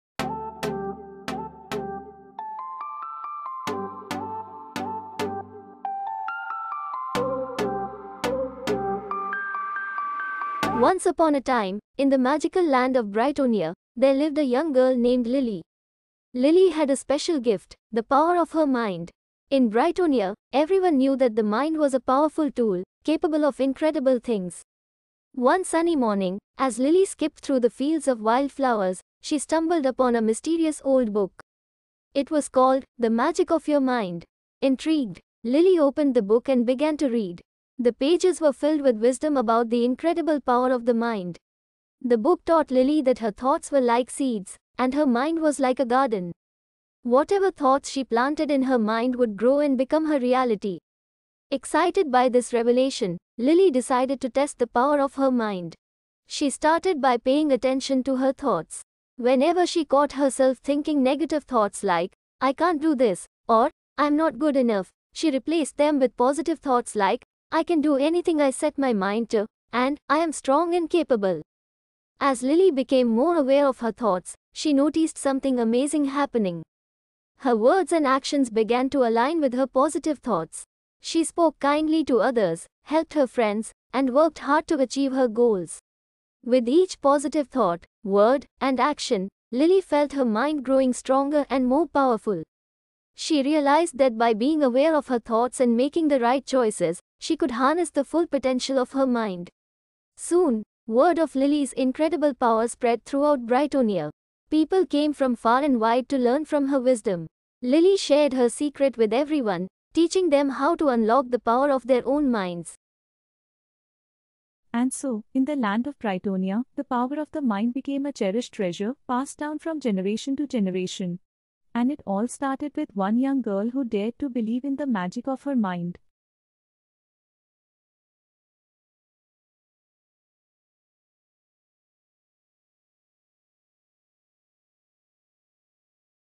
Short story on - The Power of our Mind
The power of the mind is boundless, shaping perceptions, decisions, and realities. It navigates challenges, fuels creativity, and fosters resilience. From envisioning the future to recalling past experiences, the mind weaves intricate narratives, adapts to circumstances, and drives human potential to unprecedented heights through belief, intention, and imagination.